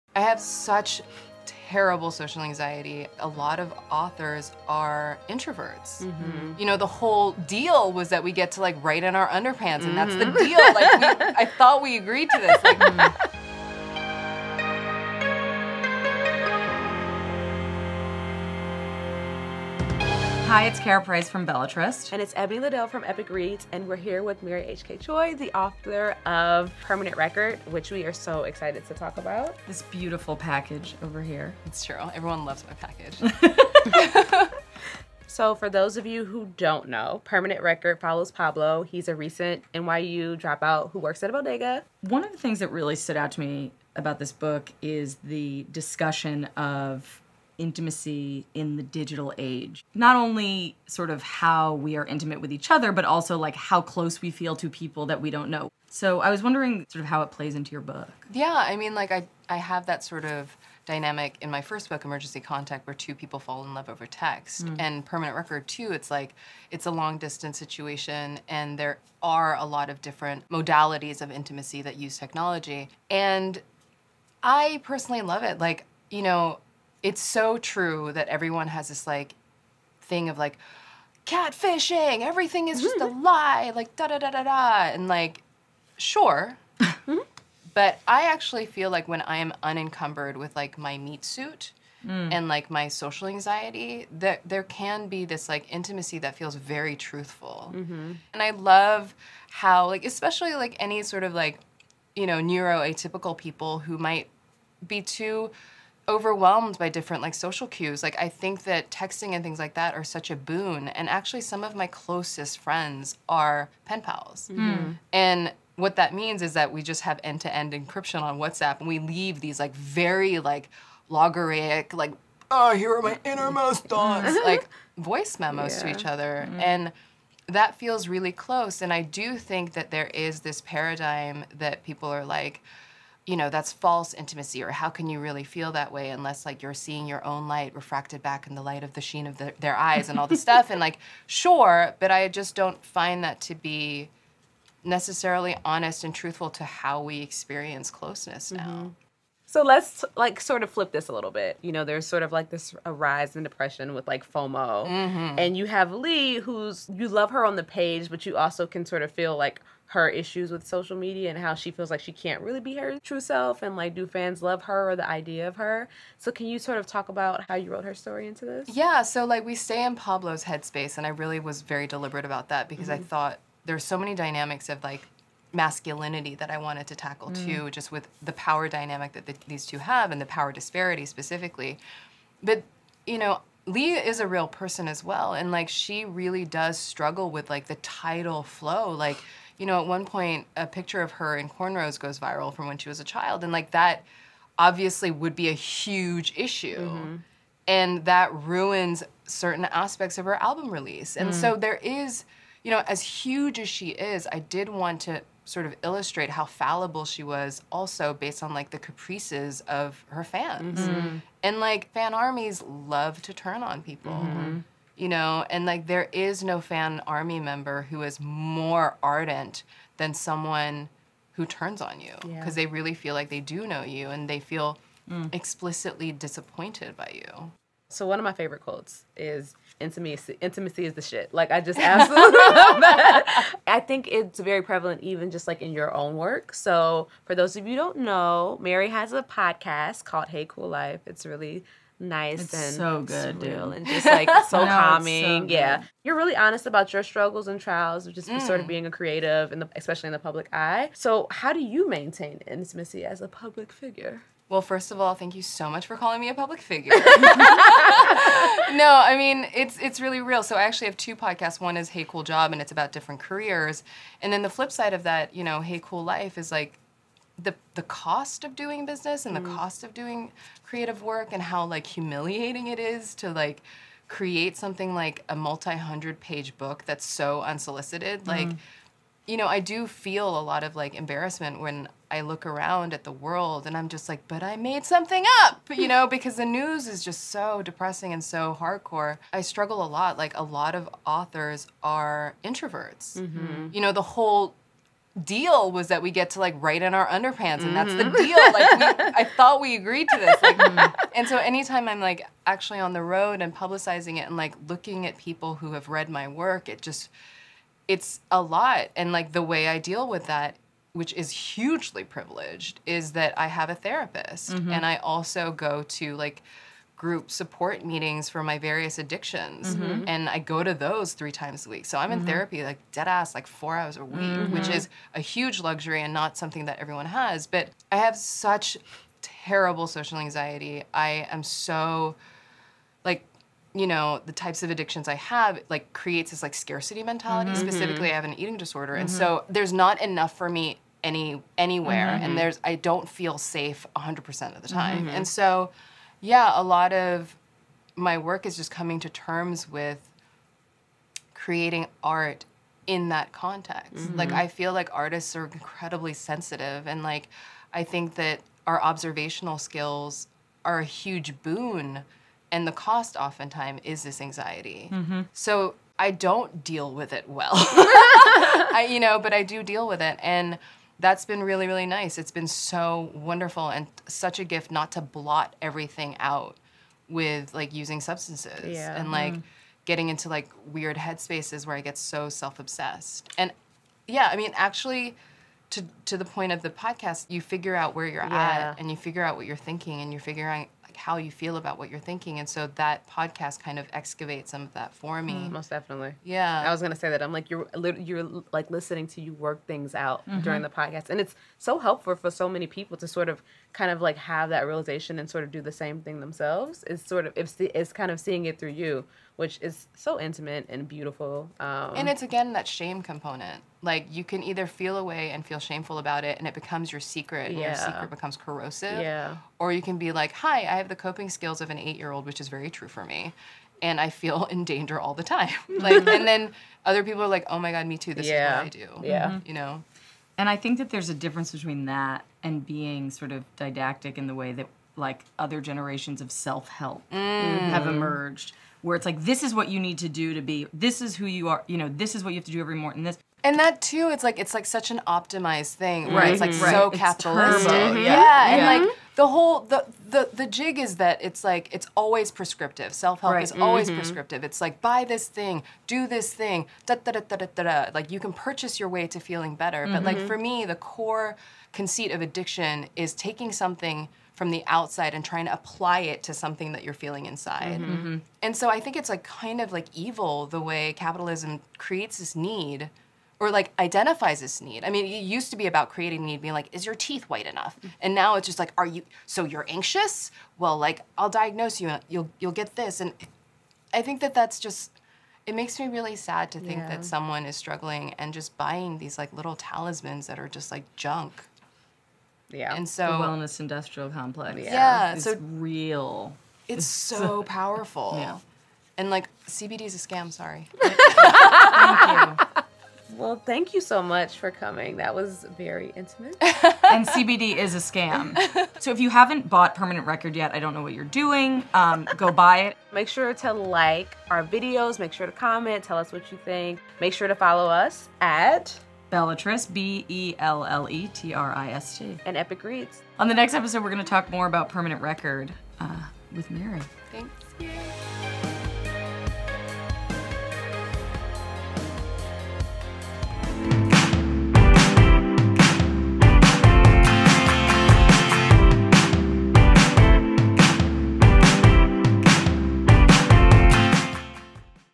Mental Health & Intimacy in the Digital Age | Permanent Record
In this episode of WhyNotYA, in which we team up with Belletrist to deep dive into our fave YA books, we talk social intimacy in the digital era with the fabulous author and public figure Mary H.K. Choi, author of Permanent Record. How does art affect the artist? How do we manage creative energy?